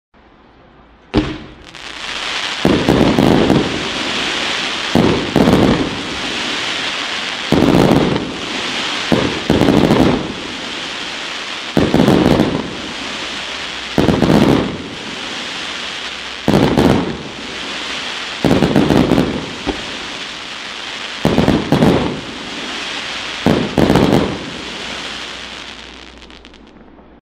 China Fireworks Supplier 1.2" 100 Shot FAN Display Cake Fireworks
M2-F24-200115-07
1.2" 100 Shot FAN Gold Crackling Tail To Blue Pistil Gold Crackling Willow